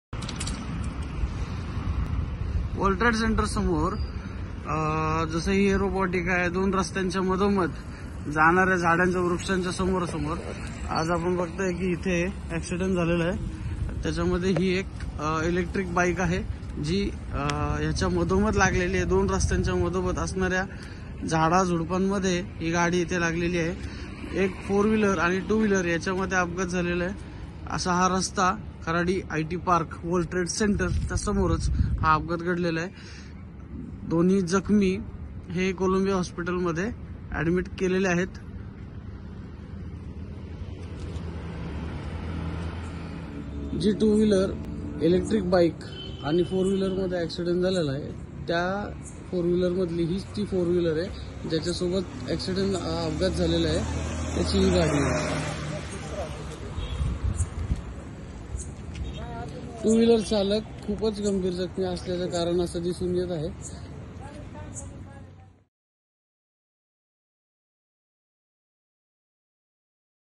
Kharadi IT Park - World Trade Center | खराडी आयटी पार्क : कार आणि इलेक्ट्रिक बाईकमध्ये भीषण अपघात..
Kharadi IT Park - World Trade Center | खराडी आयटी पार्क : कार आणि इलेक्ट्रिक बाईकमध्ये भीषण अपघात, दोघे जखमी
ही घटना काल रात्री झाली आहे.